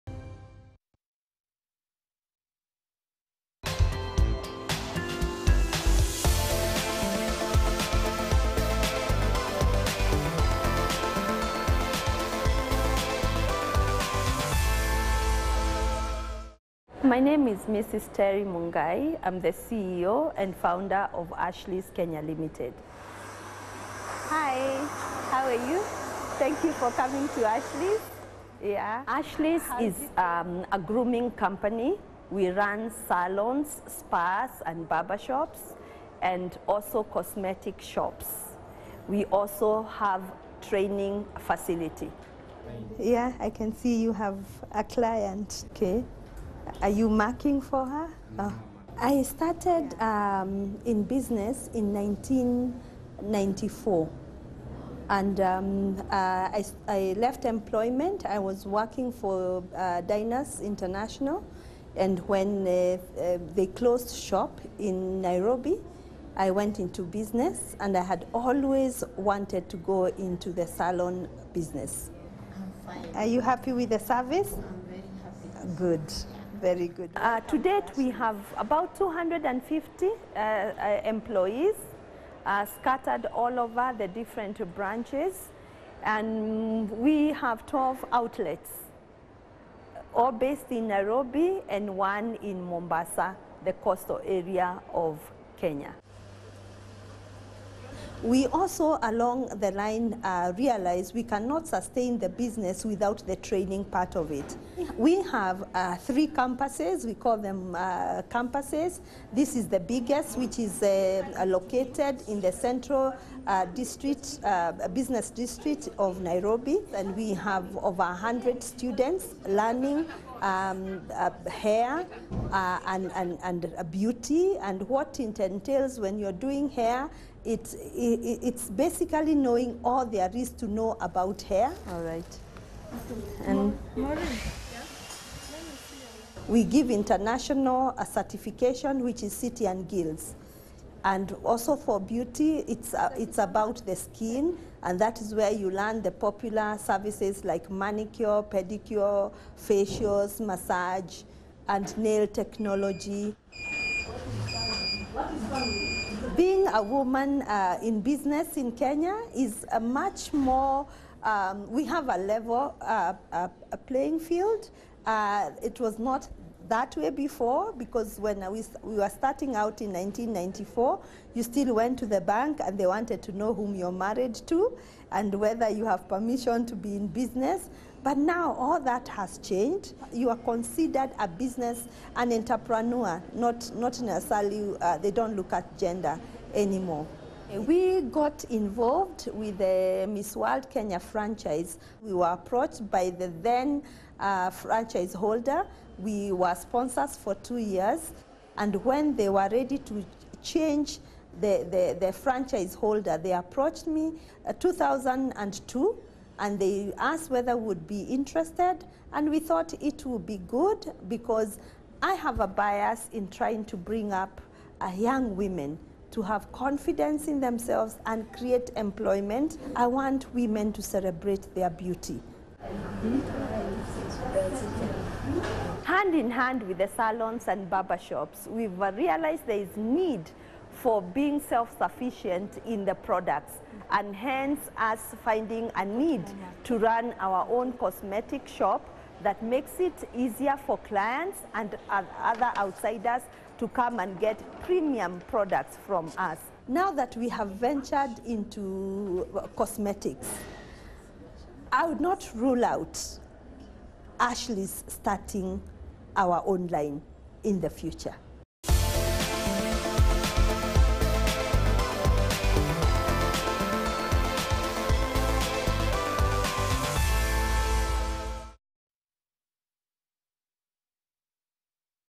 Kenya's Beauty's Ashley - BBC World News
Terry, the founder and chief executive of Ashley's Kenya, a beauty company with more than 240 employees